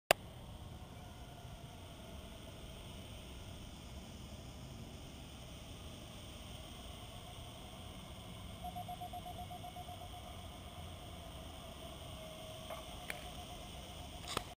Antenna QRP
Aprendiendo telegrafia.

Instalación sencilla de la antena de hilo (10mtrs 1/2 onda de 14 Mhz) con una caña de pescar de 5mtrs puesta en horizontal, sobre un cortado de la montaña.

Cabezo Pascual, en el puerto de garruchal, Murcia.